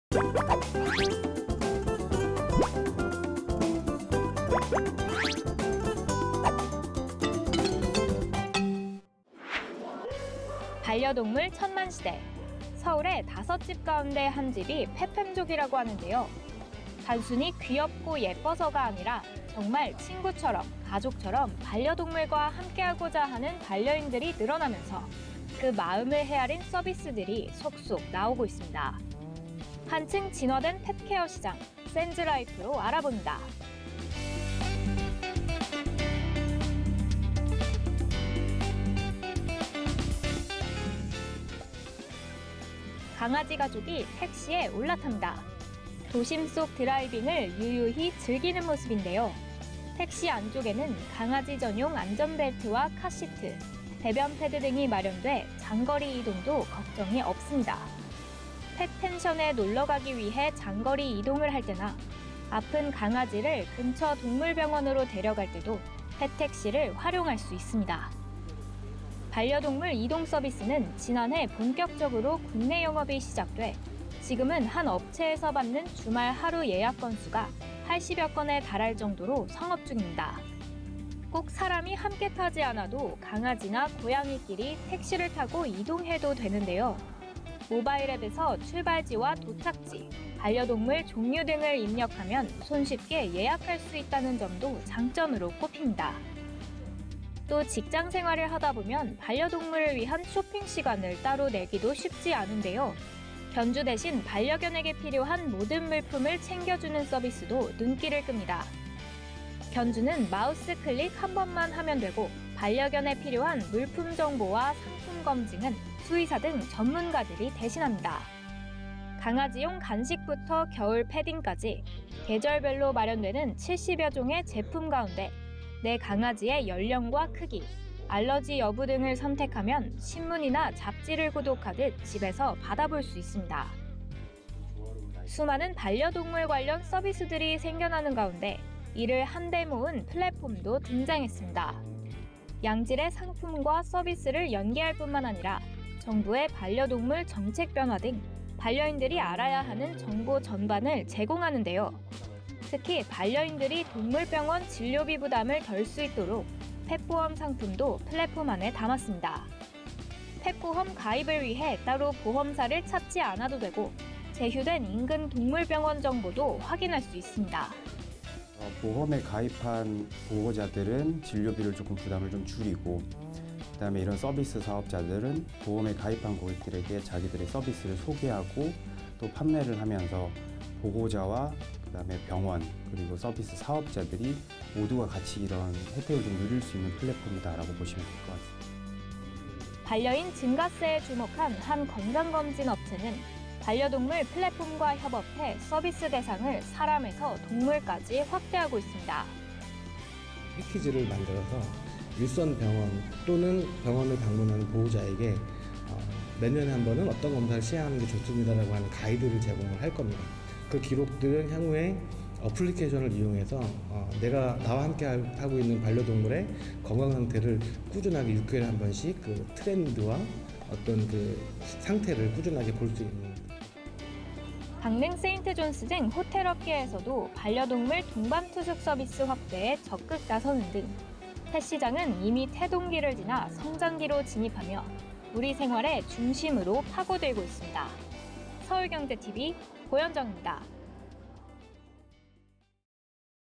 [센즈라이프] “펫택시부터 건강검진까지”…진화하는 펫케어
[인트로]
반려동물 천만시대, 서울의 다섯 집 가운데 한 집이 펫팸족이라고 하는데요.
단순히 귀엽고 예뻐서가 아니라, 정말 친구처럼 가족처럼 반려동물과 함께 하고자 하는 반려인들이 늘어나면서, 그 마음을 헤아린 서비스들이 속속 나오고 있습니다.
한층 진화된 펫케어(PetCare) 시장, 센즈라이프로 알아봅니다.

[기자]
강아지 가족이 택시에 올라탑니다.

도심 속 드라이빙을 유유히 즐기는 모습인데요.

택시 안쪽에는 강아지 전용 안전벨트와 카시트, 배변패드 등이 마련돼 장거리 이동도 걱정이 없습니다.

펫팬션에 놀러가기 위해 장거리 이동을 할 때나 아픈 강아지를 근처 동물병원으로 데려갈 때도 펫택시를 활용할 수 있습니다.

반려동물 이동서비스는 지난해 본격적으로 국내 영업이 시작돼, 지금은 한 업체에서 받는 주말 하루 예약 건수가 80여건에 달할 정도로 성업중입니다.

꼭 사람이 함께 타지 않아도, 강아지나 고양이끼리 택시를 타고 이동해도 되는데요.
모바일 앱에서 출발지와 도착지, 반려동물 종류 등을 입력하면 손쉽게 예약할 수 있다는 점도 장점으로 꼽힙니다.

또 직장 생활을 하다 보면 반려동물을 위한 쇼핑 시간을 따로 내기도 쉽지 않은데요.
견주 대신 반려견에게 필요한 모든 물품을 챙겨주는 서비스도 눈길을 끕니다.

견주는 마우스 클릭 한 번만 하면 되고, 반려견에 필요한 물품 정보와 상품 검증은 수의사 등 전문가들이 대신합니다.

강아지용 간식부터 겨울 패딩까지 계절별로 마련되는 70여종의 제품 가운데, 내 강아지의 연령과 크기, 알러지 여부 등을 선택하면 신문이나 잡지를 구독하듯 집에서 받아볼 수 있습니다.

수많은 반려동물 관련 서비스들이 생겨나는 가운데, 이를 한데 모은 플랫폼도 등장했습니다.

양질의 상품과 서비스를 연계할 뿐만 아니라, 정부의 반려동물 정책 변화 등 반려인들이 알아야 하는 정보 전반을 제공하는데요.
특히 반려인들이 동물병원 진료비 부담을 덜 수 있도록 ‘펫보험’ 상품도 플랫폼 안에 담았습니다.
펫보험 가입을 위해 따로 보험사를 찾지 않아도 되고, 제휴된 인근 동물병원 정보도 확인할 수 있습니다.

[인터뷰] 이수민 / ‘더 펫허그’ 사업팀장
“보험에 가입한 보호자들은 진료비 부담을 줄이고, 서비스 사업자들은 보험에 가입한 고객들에게 자신들의 서비스를 소개하고 또 판매하면서, 보호자와 병원, 그리고 서비스 사업자들이 모두가 같이 혜택을 누릴 수 있는 플랫폼입니다.”

반려인 증가세에 주목한 한 건강검진 업체는 반려동물 플랫폼과 협업해 서비스 대상을 사람에서 동물까지 확대하고 있습니다.

[인터뷰] 김상도 / (주)헬로 대표
“(건강검진) 패키지를 만들어서 일선 병원, 또는 병원을 방문하는 보호자에게 ‘몇 년에 한번은 어떤 검사를 시행하는 게 좋습니다’라고 하는 가이드를 제공할 겁니다. 그 기록들은 향후에 어플리케이션을 이용해서 내가 나와 함께하고 있는 반려동물의 건강 상태를 꾸준하게 6개월에 한 번씩 트렌드와 상태를 꾸준하게 볼 수 있는…”

강릉 세인트존스 등 호텔업계에서도 반려동물 동반 투숙 서비스 확대에 적극 나서는 등 펫시장은 이미 태동기를 지나 성장기로 진입하며 우리 생활의 중심으로 파고들고 있습니다. 서울경제TV 고현정입니다.

[영상취재 오성재, 김경진 / 영상편집 이한얼]